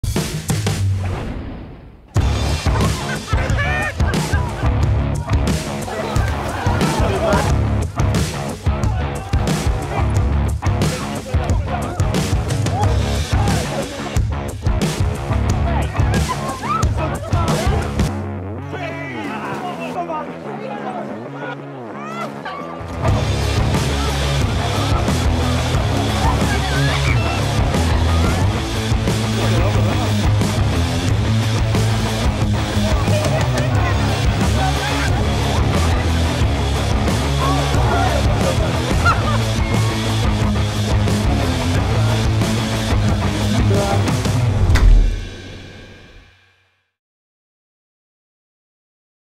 Block Ness Monster by So.Beer | Tough Mudder
Brought to you in 2021 by So.Beer, we loved watching thousands of Mudders go through the Block Ness Monster spin cycle. Reminisce on your own wrestle with the beast in our brand new Block Ness Monster highlight reel.